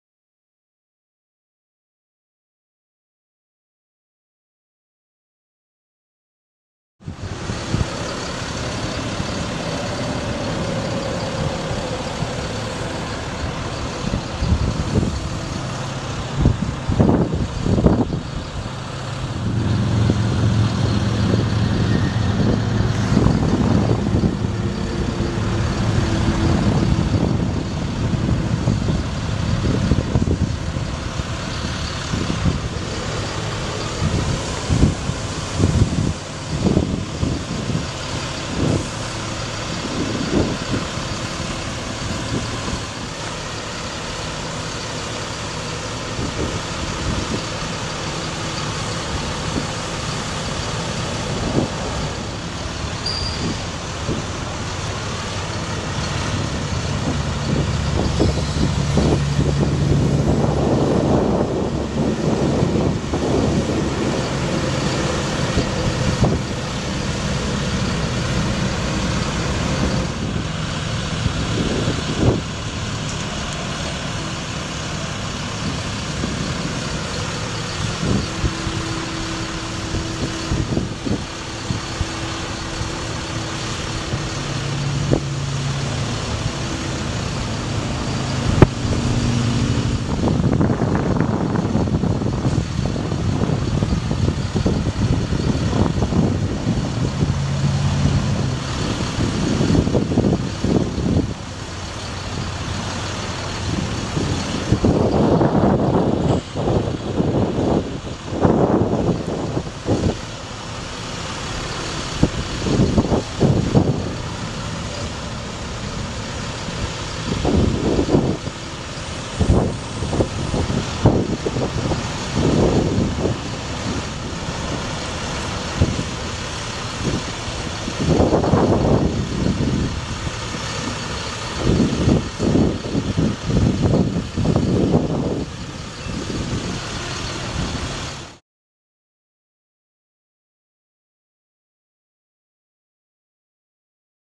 Versalift VN50 Bucket Truck For Sale - 13021
Versalift VN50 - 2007 Ford F750 Bucket Truck for sale at Utility Fleet! LOW MILEAGE!